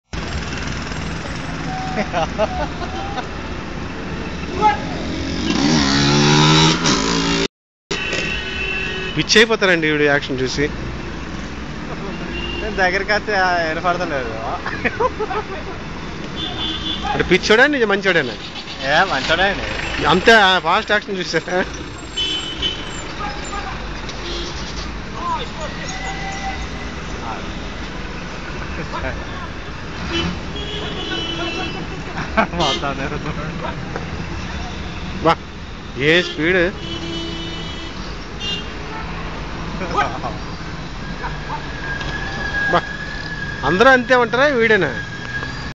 traffic control with dance
Traffic control in shillong in different style never seen earlier